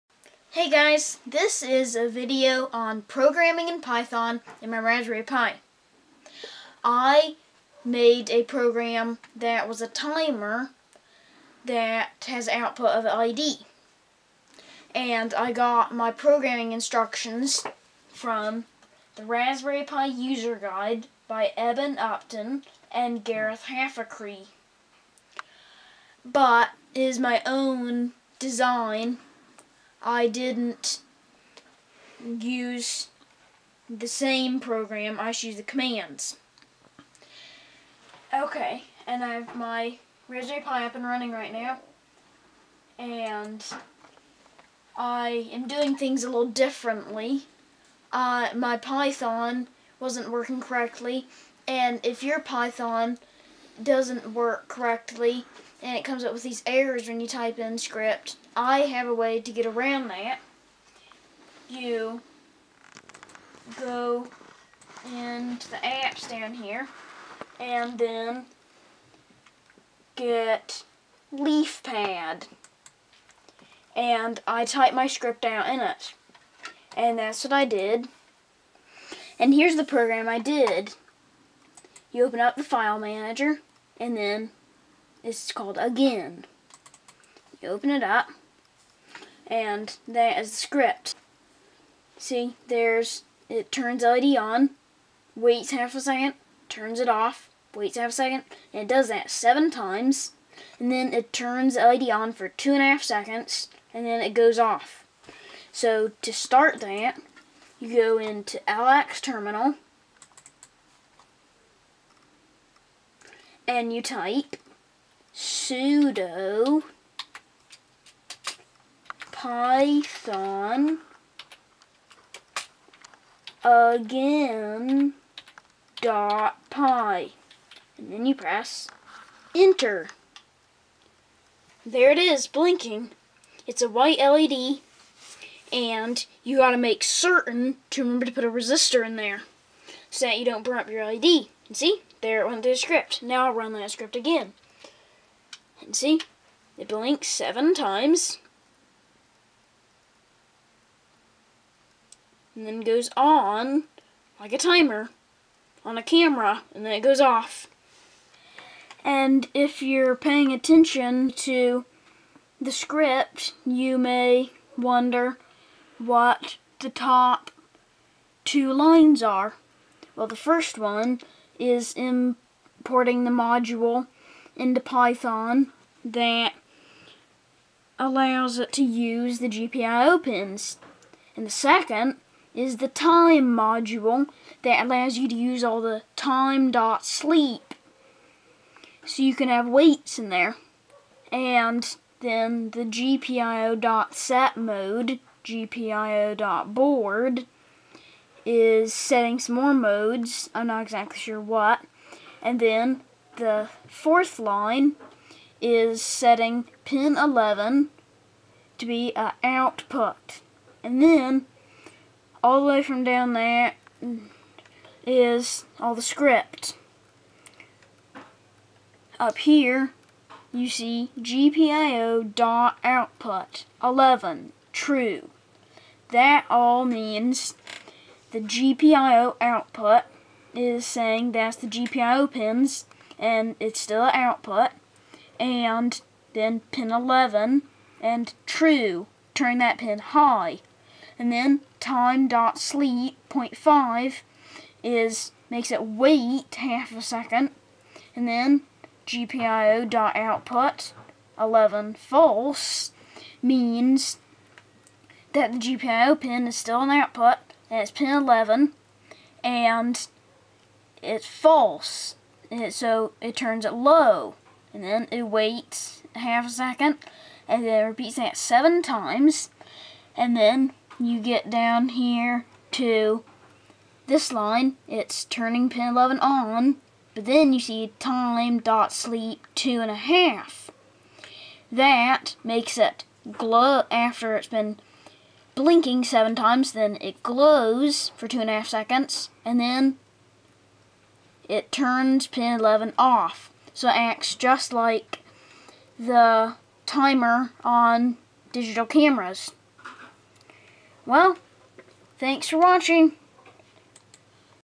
An instructional tutorial on a Raspberry Pi led timer I made in Python
Python programing on the Raspberry pi B.  I made an LED circuit blink using a Python timer program I made.